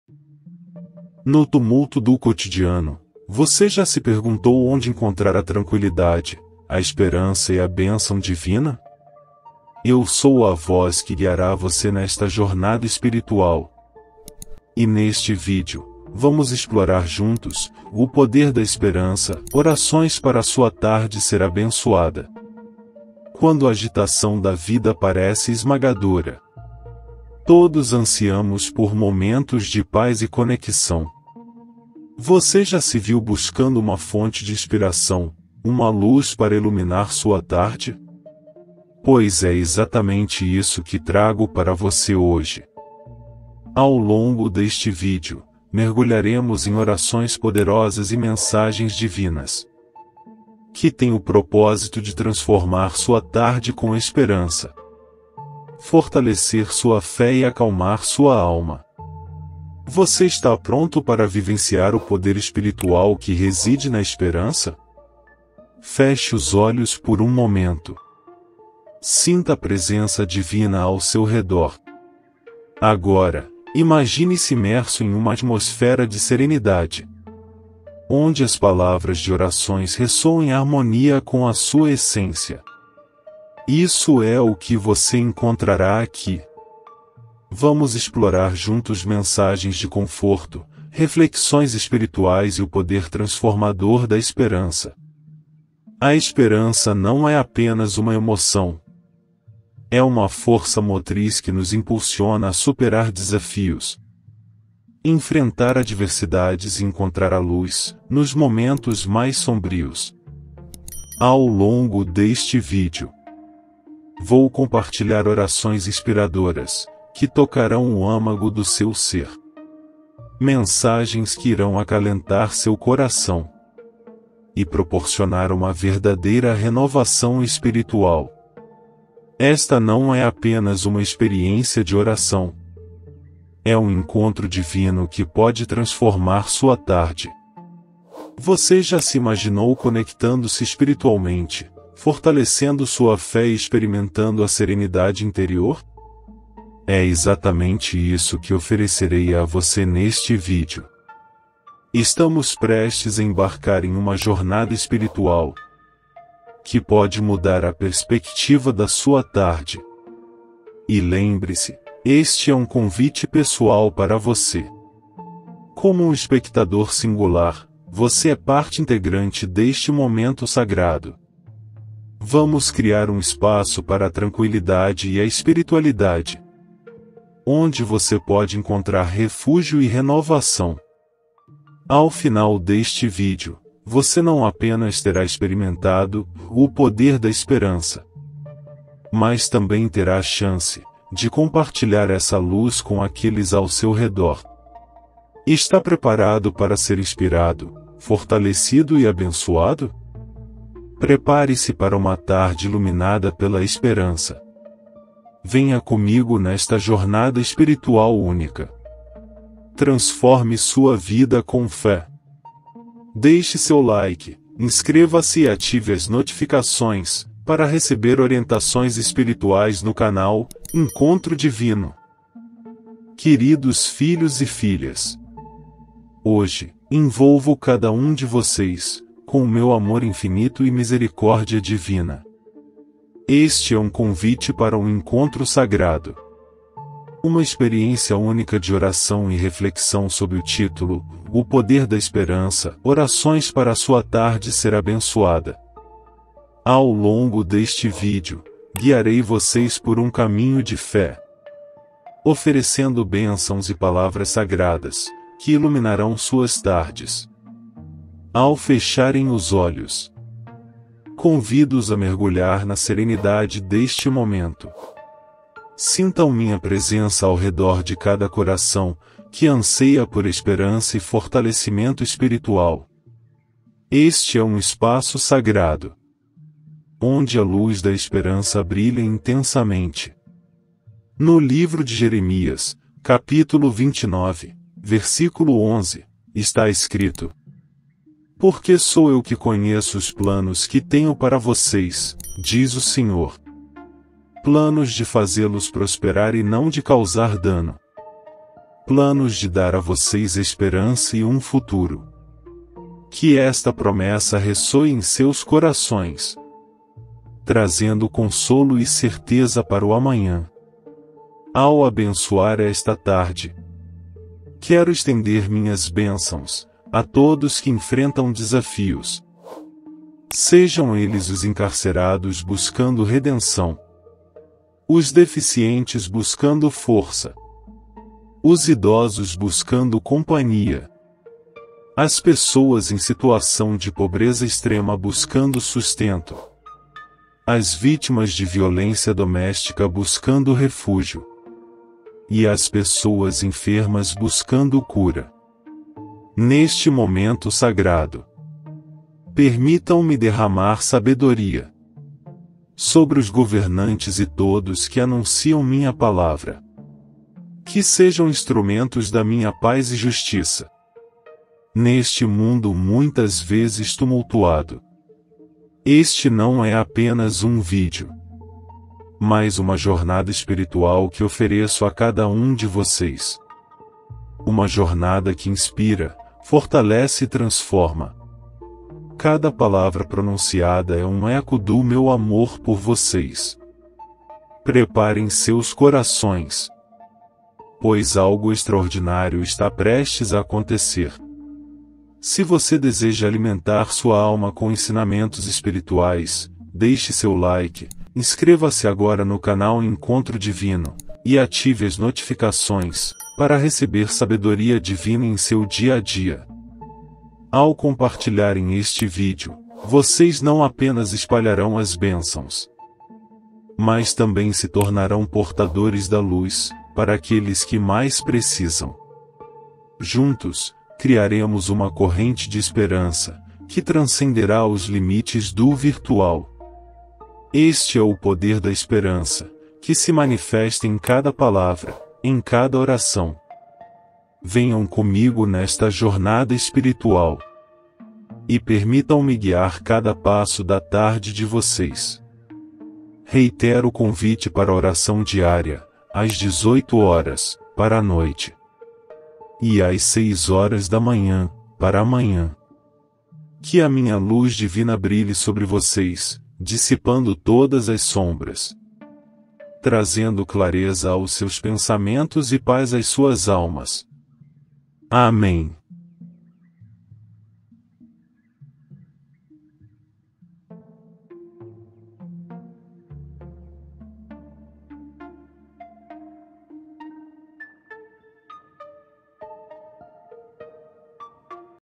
O PODER DA ESPERANÇA: ORAÇÕES PARA SUA TARDE SER ABENÇOADA!
"Sinfonia Celestial: O Poder da Oração e Mensagens Transformadoras"

Bem-vindos à jornada espiritual que transformará sua tarde! 🌅 Em "O Poder da Esperança: Orações para sua Tarde Ser Abençoada!", convido vocês a uma experiência única de conexão divina. Neste espaço sagrado, mergulharemos juntos na serenidade e nas bênçãos que fortalecerão seus corações.

✨ Sintam-se abraçados pelo amor infinito e misericórdia divina enquanto exploramos passagens bíblicas, como Jeremias 29:11, que prometem um futuro cheio de esperança. Este não é apenas um vídeo, mas um convite para renovar sua fé e encontrar conforto nos desafios da vida.

Ao longo desta oração, direciono minhas bênçãos a cada um de vocês, e também às almas que enfrentam diferentes lutas. Que estas palavras alcancem os encarcerados em busca de redenção, os deficientes em busca de força, os idosos em busca de companhia, aqueles em situação de pobreza extrema em busca de sustento, as vítimas de violência doméstica em busca de refúgio e as pessoas enfermas em busca de cura.

Este vídeo é mais do que uma mensagem; é uma jornada espiritual que oferece mensagens de força, fé e coragem. Cada palavra pronunciada é um reflexo do amor divino, uma luz em meio às sombras.

Ao compartilharem este momento sagrado, tornam-se portadores da esperança para aqueles que mais precisam. Criaremos juntos uma corrente de amor e fé que transcenderá as fronteiras do virtual.

Preparem-se para uma experiência transformadora, guiada pela luz divina. 🌟 Ao final, convido todos a se unirem à oração diária às 18 horas, para a noite, e às 6 horas da manhã, para a manhã.

Juntos, espalharemos o "O Poder da Esperança" e construiremos um mundo mais iluminado. 🙏